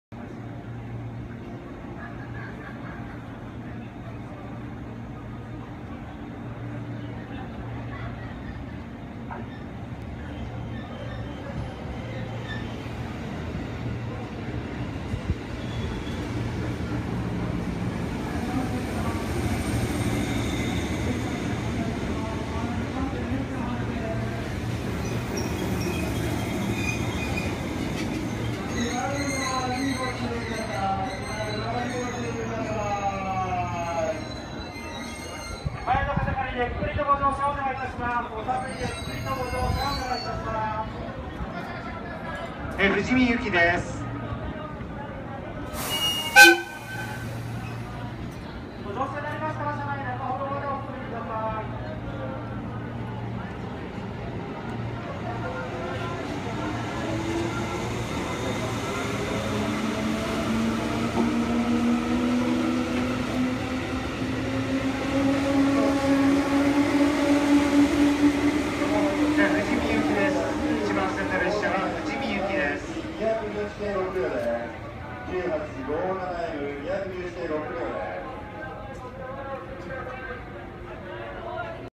【JR東日本】JR上諏訪駅 211系中央本線普通列車の発車シーン
諏訪湖花火大会後の臨時列車で、中央本線で増備が続く211系普通列車の発車シーンです。空笛あり。